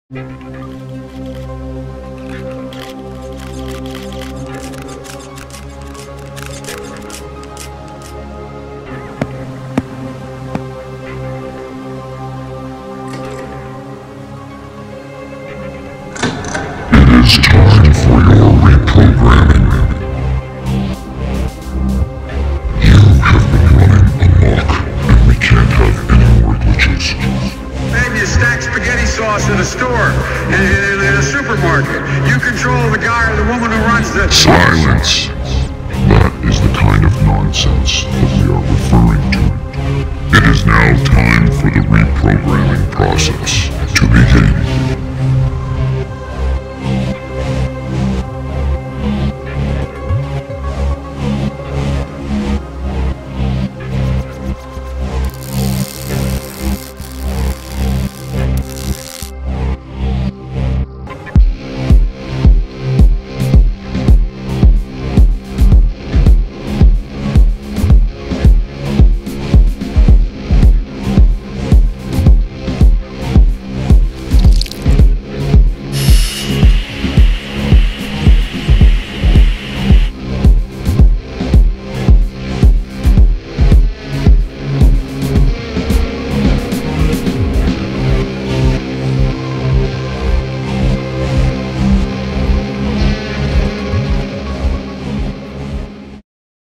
president joe's transformation
President joe's transformation . President Joe biden has been malfunctioning! The president’s glitches have gotten out of control & the Illuminati corrects the cyborg president with a weird transformation process.

Concept & storyline credit: James Sampson